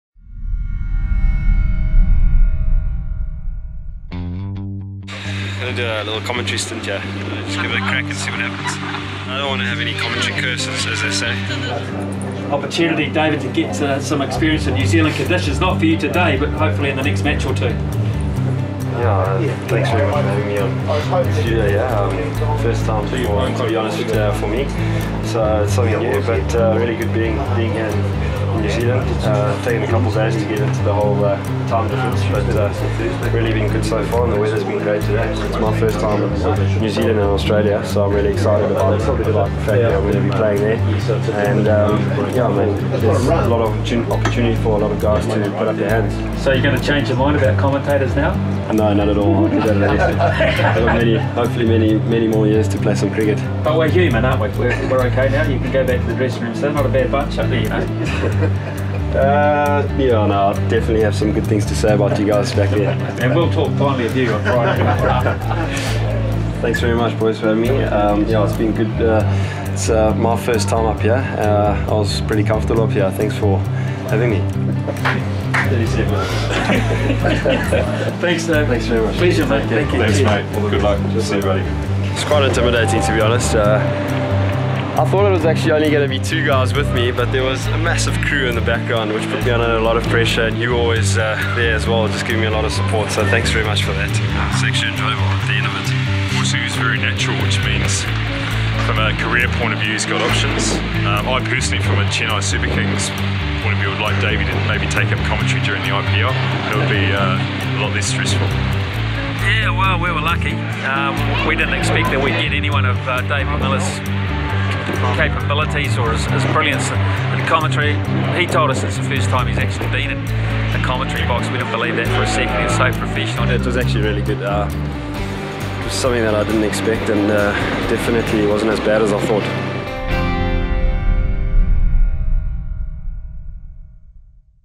NZ Tour Diaries Eps 4 - Miller on the Mic
Professional cricketers often stay involved in the game after their playing days by getting involved in commentary. But Proteas batsman David Miller got involved in a bit of commentary during his the first ODI against New Zealand! Take a behind the scenes look at what he had to say when he joined cricket legend Stephen Fleming and the voice of New Zealand cricket, Ian Smith, in the Sky Sports commentary box...